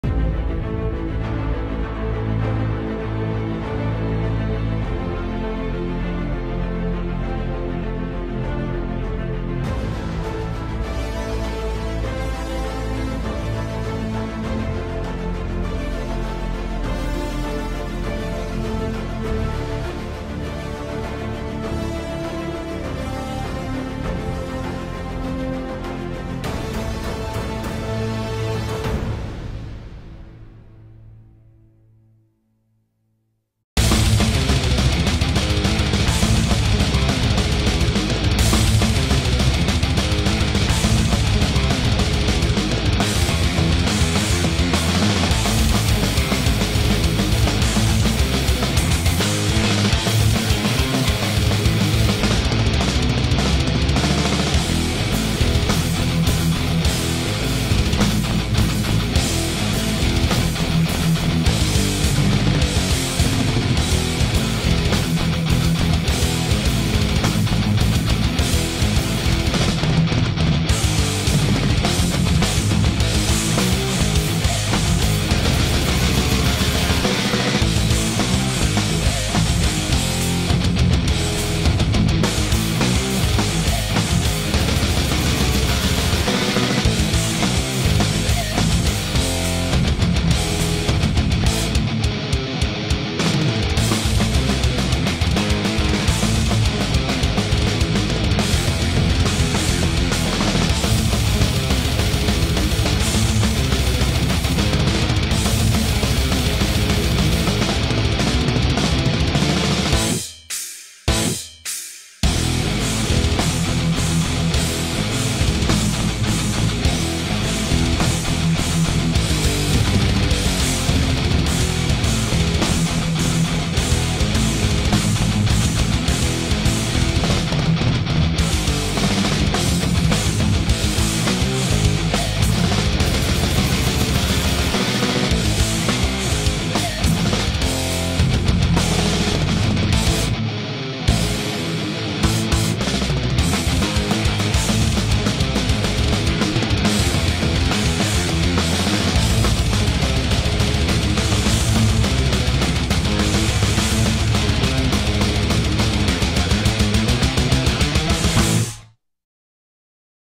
JL Skinner Boise State Safety! Best in the West!🔥🔥🦾 2021 Highlights
For a complete preview of the 2022 Boise State secondary watch this video I did with former BSU safety Kekoa Nawahine!